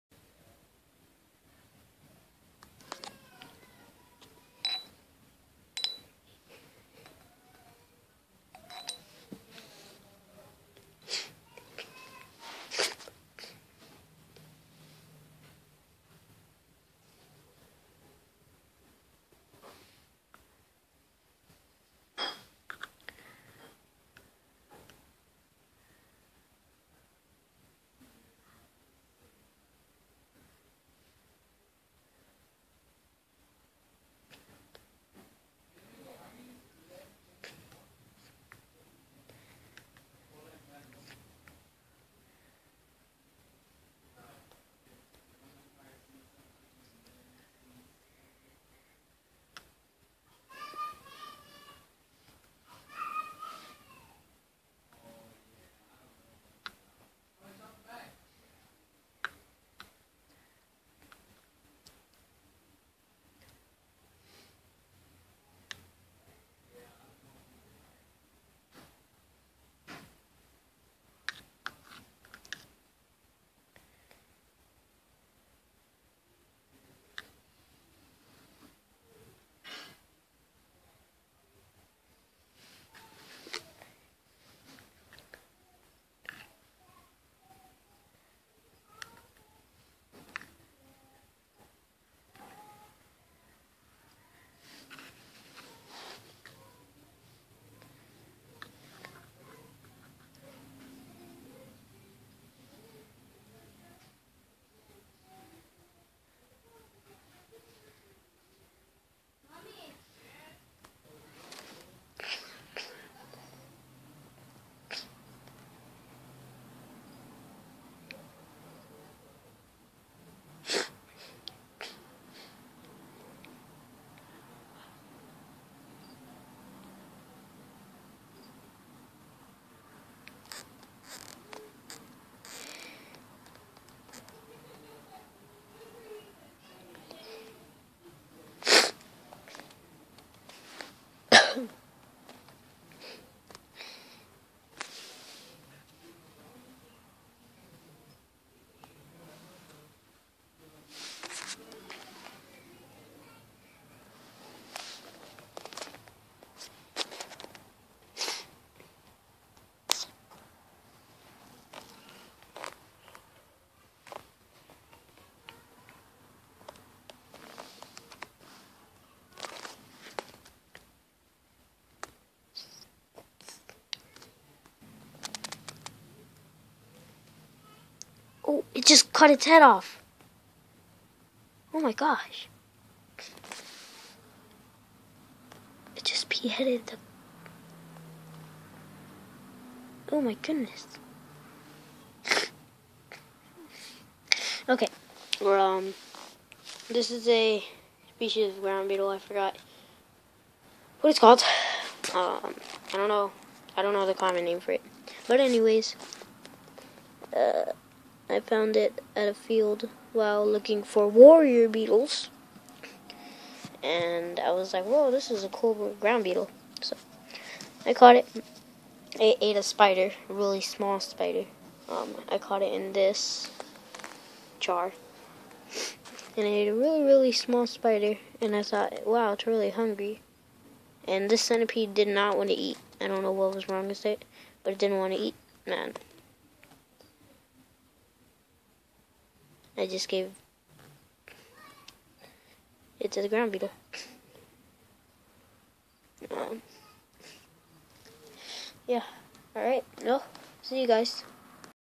Lithobius Forficatus eaten by Pterostichus Beetle
This is a Pterostichus ground beetle species I found while invert hunting. One of my garden centipedes (Lithobius Forficatus) wasn't eating. So, the pede became food. Don't worry, I have 4 more pedes.